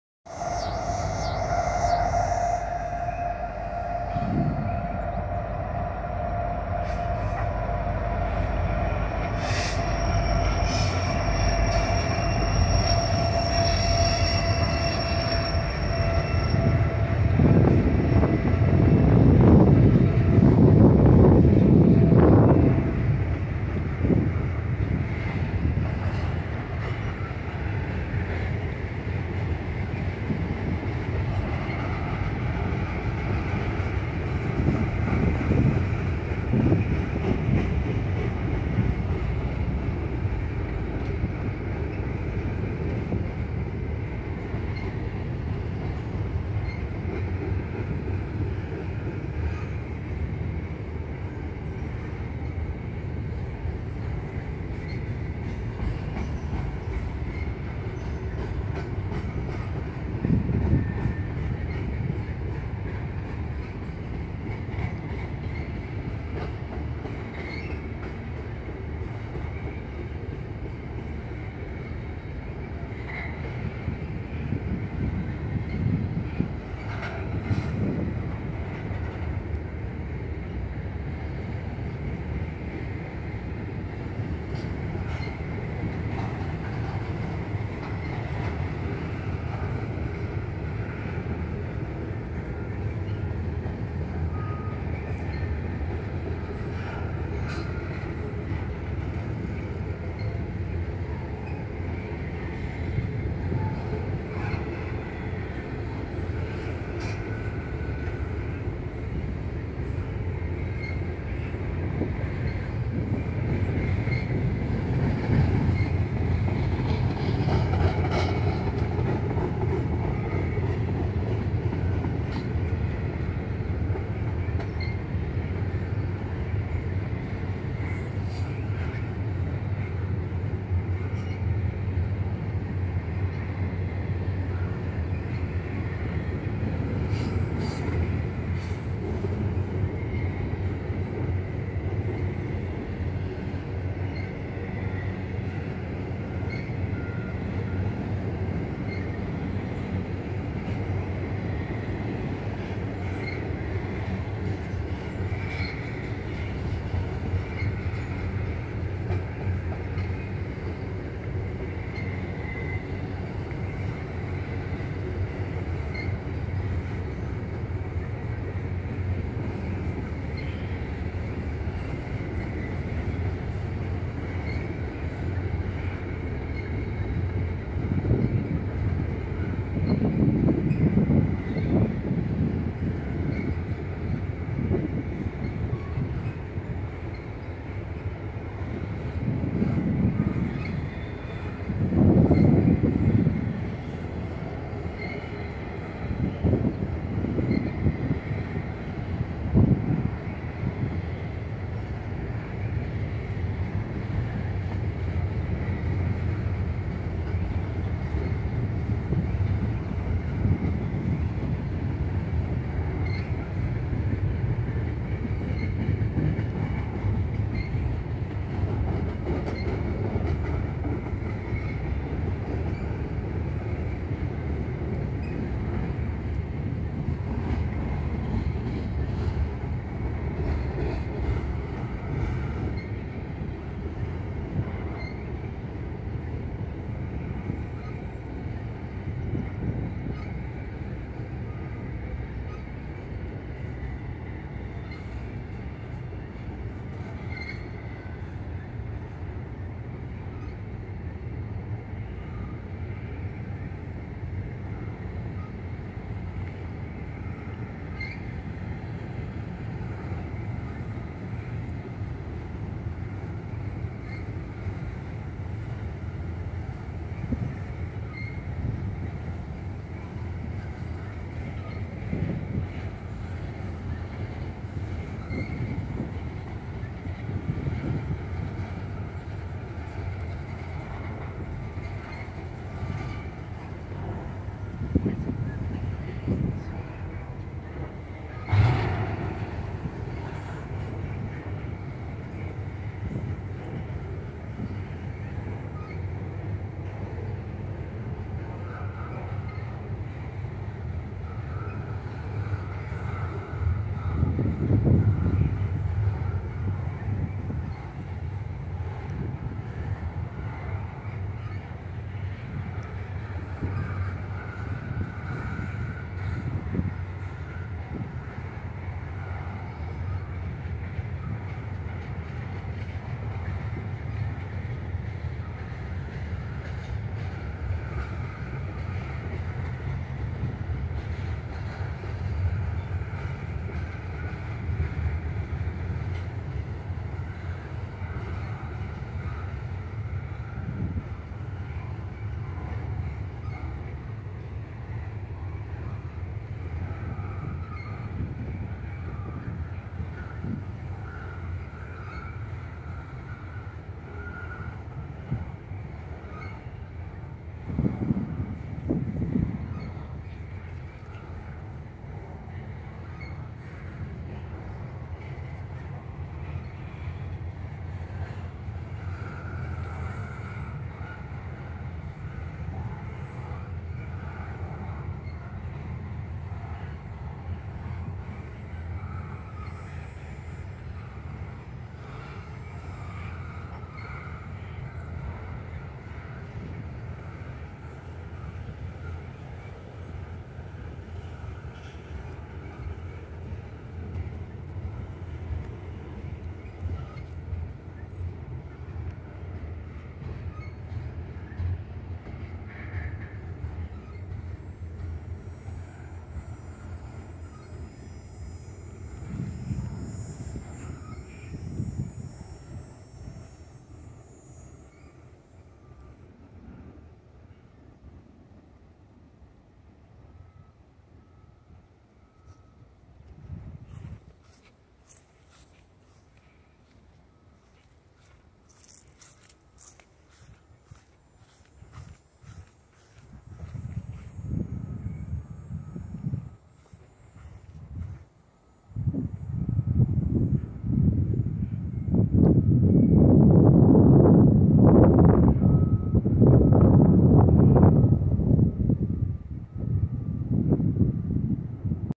CN Intermodal Train Going Slow at the Bartlett Park, IL on a nice day.
This CN Train was going slow because it was probably letting the other CN Train passing the other direction or the same way. I hope you folks enjoy this CN Intermodal Train going slow.